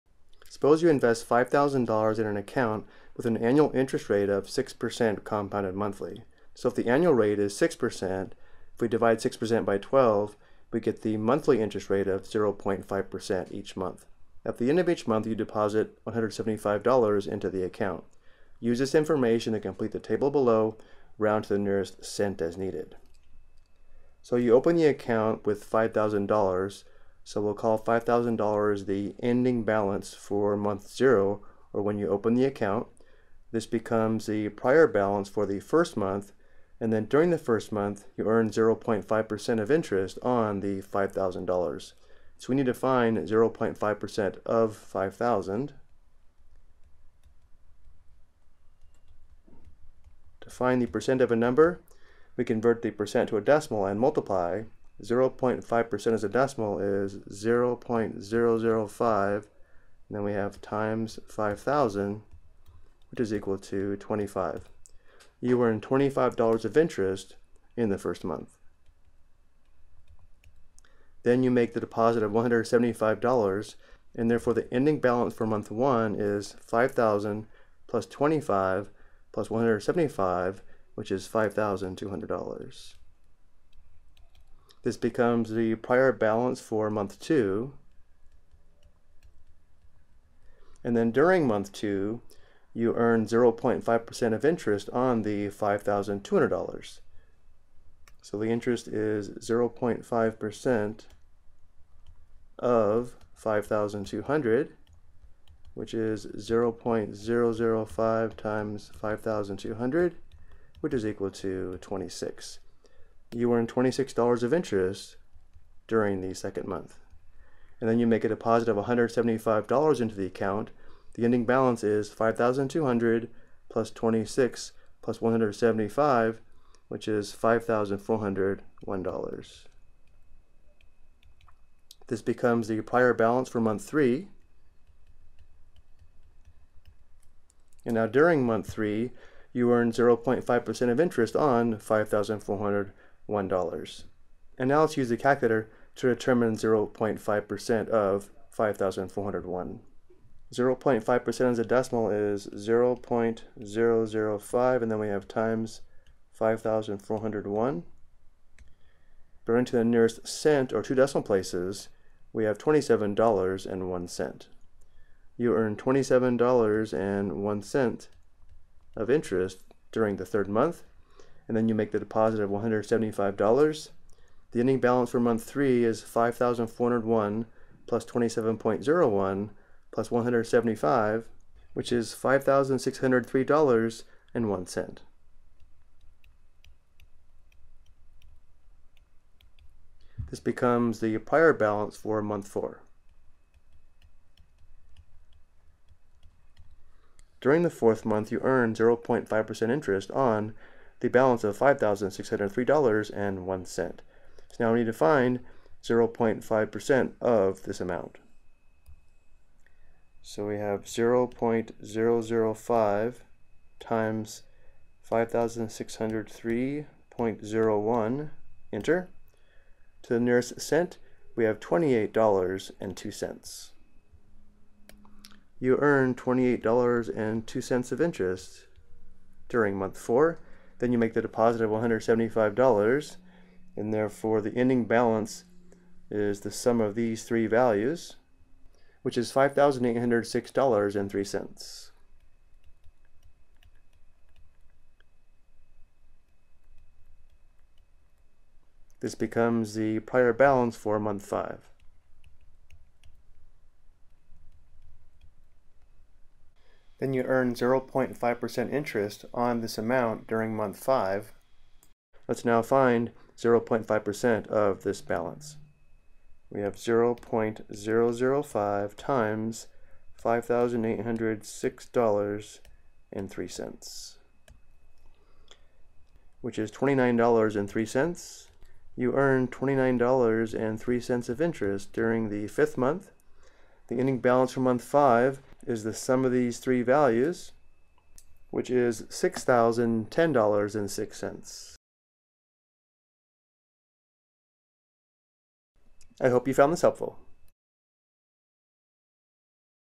Complete a Saving Table with Monthly Deposits and Compounded Interest
This video show show to complete a table showing 5 months of a savings account balance with monthly deposits and monthly compounded interest.